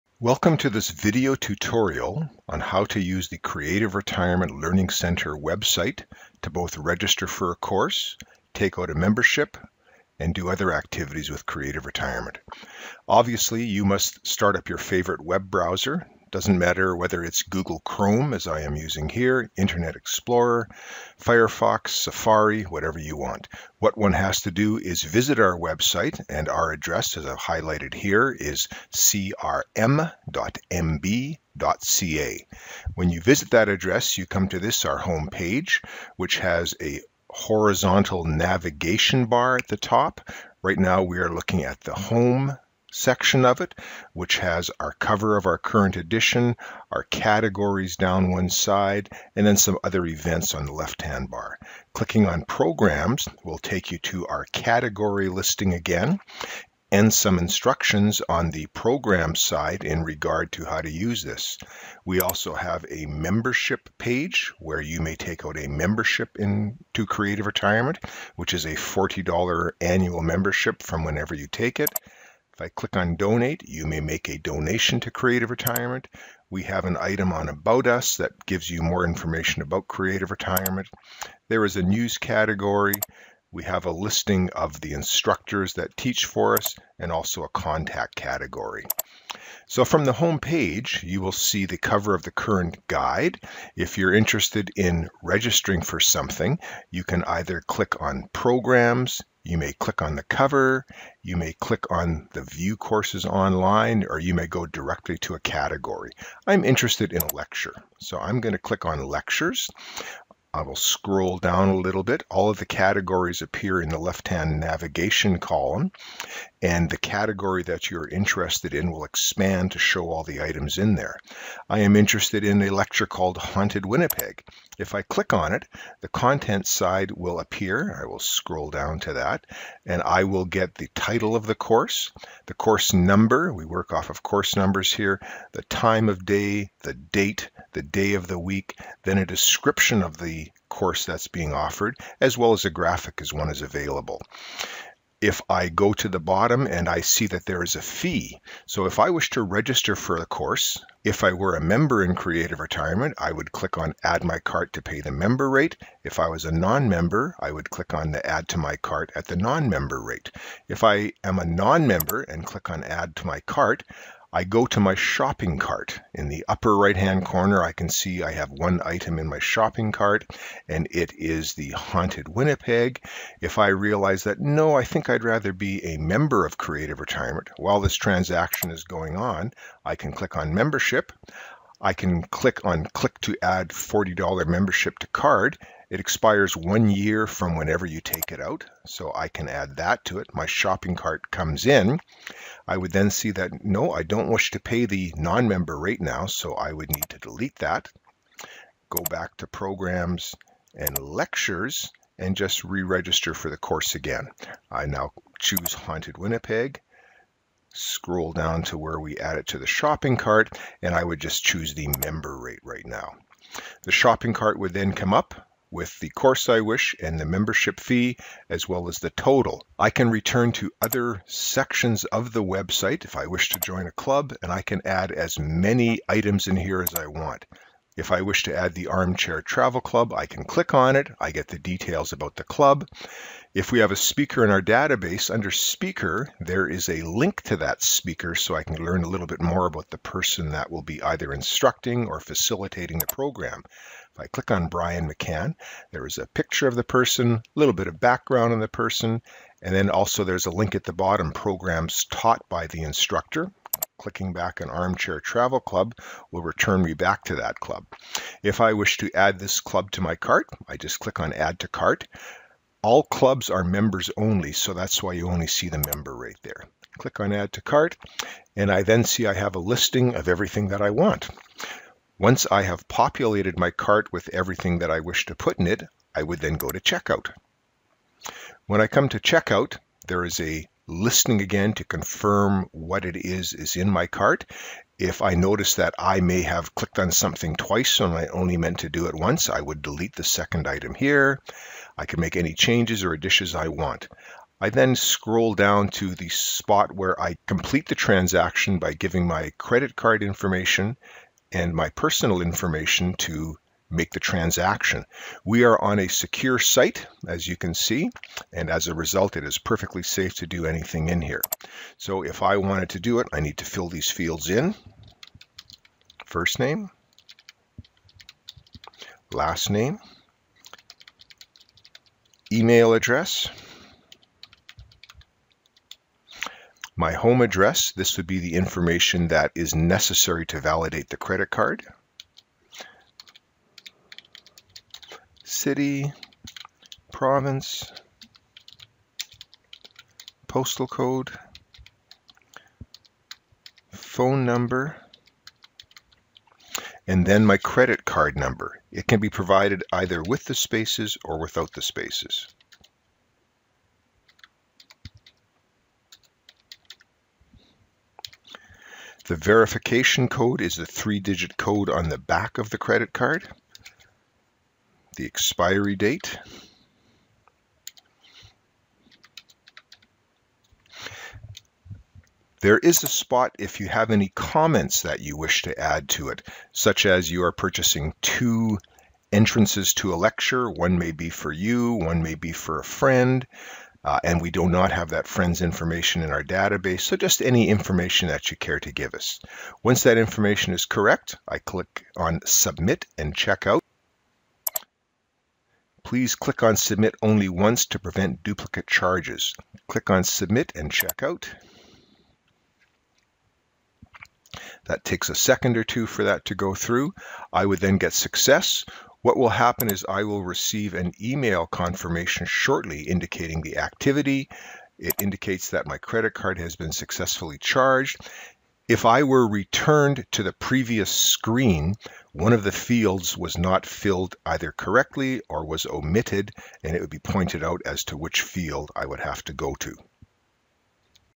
CRM Website Registration
How to register for classes, courses, renew or take out a membership, make a donation and learn more about Creative Retirement Learning Centre.